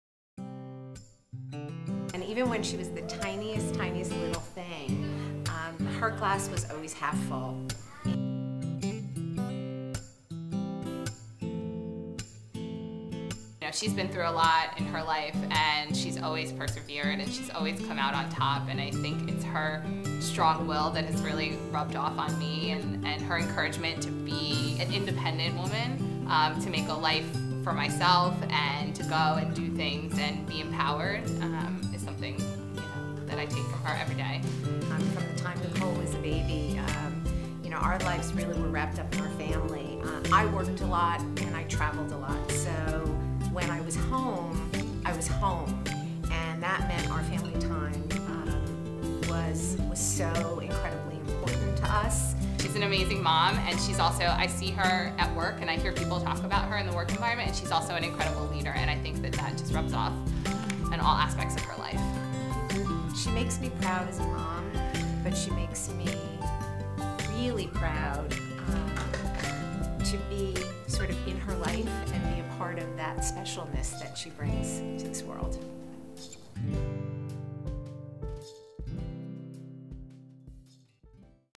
White House Black Market Sits Down with Brand President Donna Noce and her Daughter, Nicole
We sat down with WHBM president Donna Noce and her Daughter, Nicole, for a lovely interview about the beautiful relationship between mother and daughter.